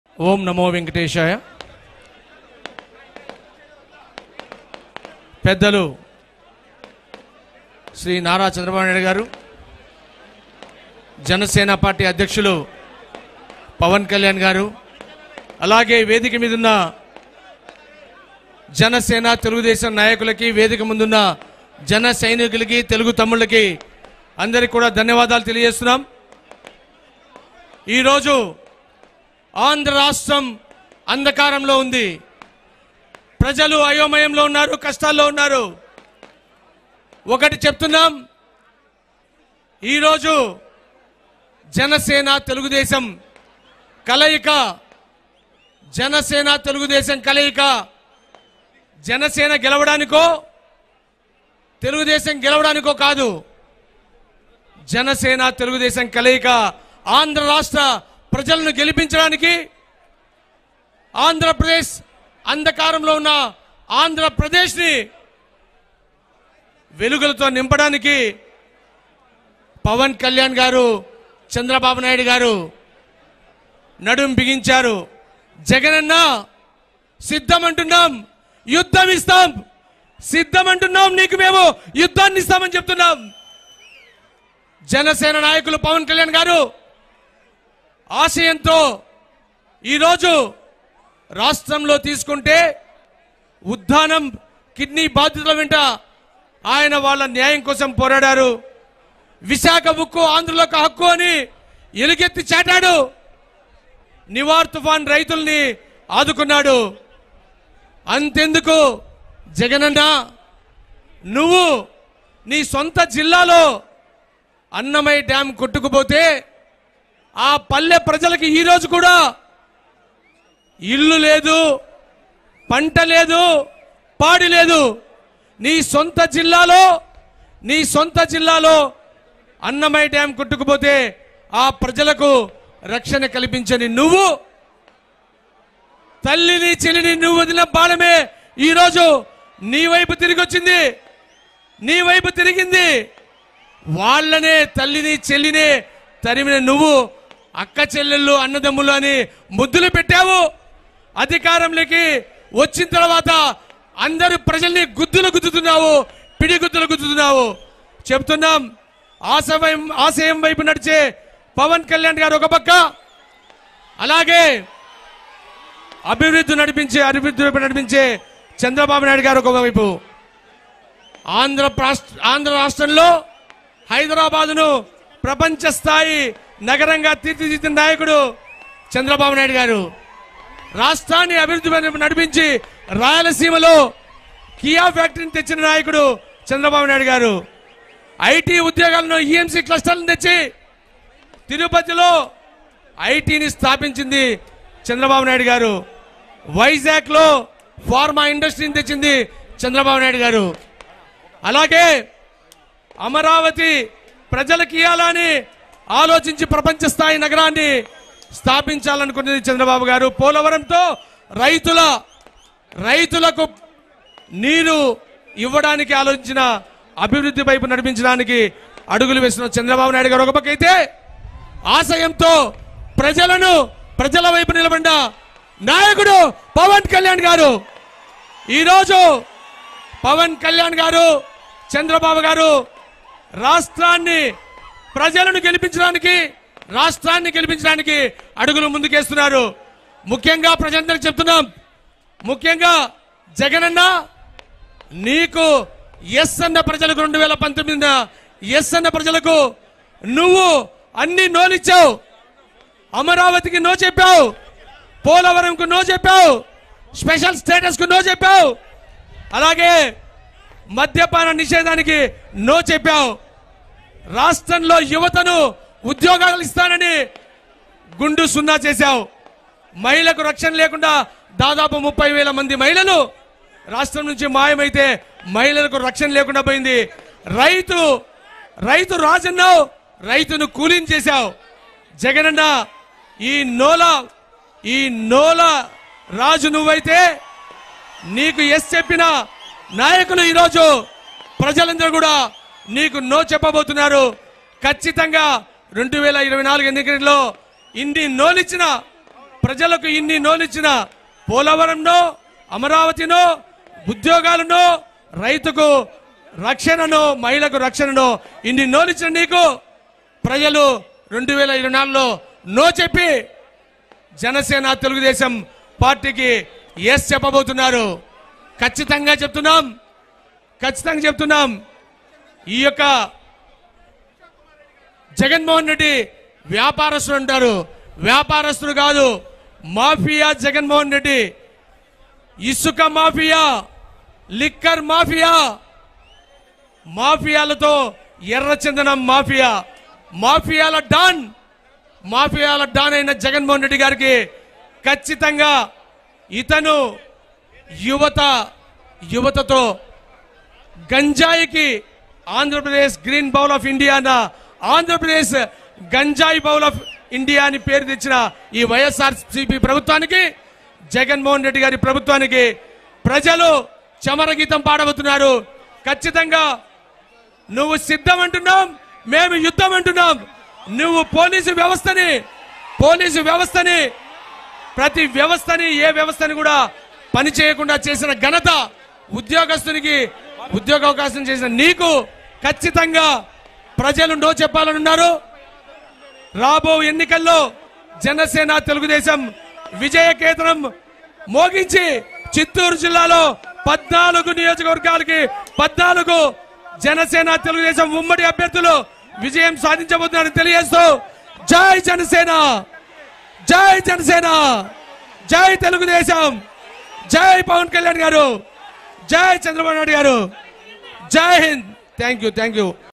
‎@i7tvlive పీలేరు రా..కదలిరా.. సభలో పసుపులేటి హరిప్రసాద్ ప్రసంగం | TDP | Chandrabau | Pawan Kalyan
పీలేరు రా..కదలిరా.. సభలో పసుపులేటి హరిప్రసాద్ గారి ప్రసంగం | TDP | Chandrabau | Pawan Kalyan